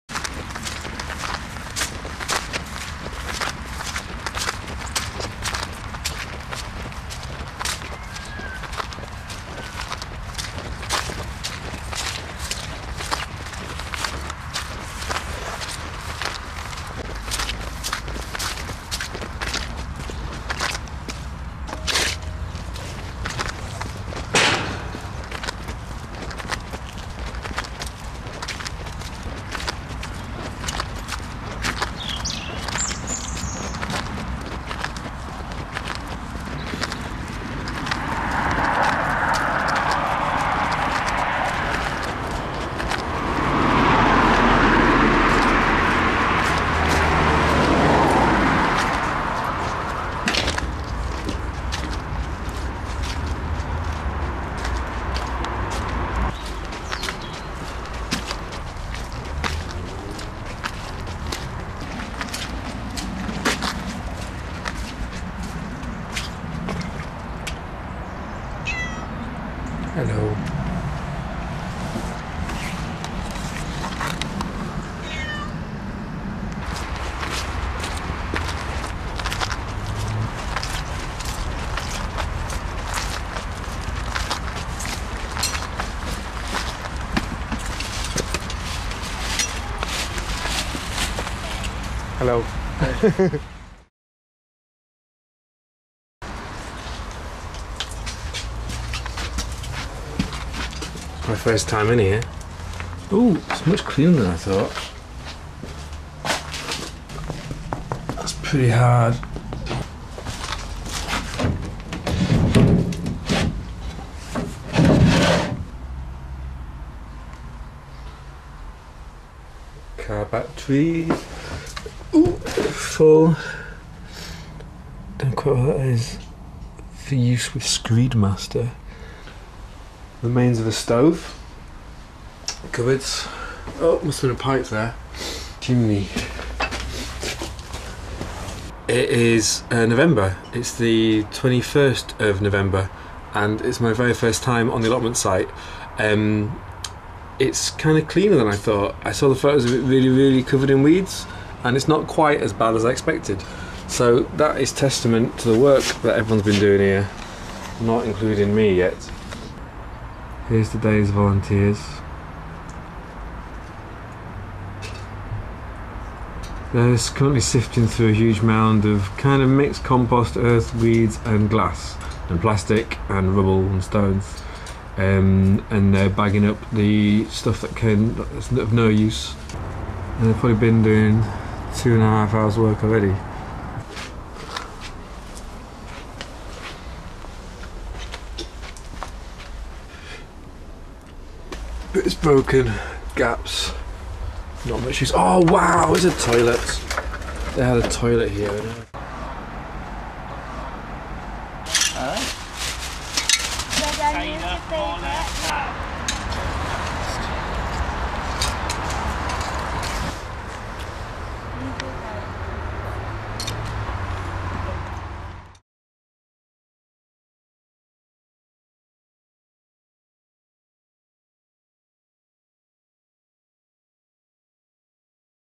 SCAN Allotment 1 My 1st visit.mpg
Walking across the moors and parks to find the new SCAN allotment, where volunteers were busy at work clearing out the rubble.
Then a tour of the structure - call it a shed, call it a greenhouse or a coldframe or what you will.
Features one cat, one toilet, and many many bags of rubbish.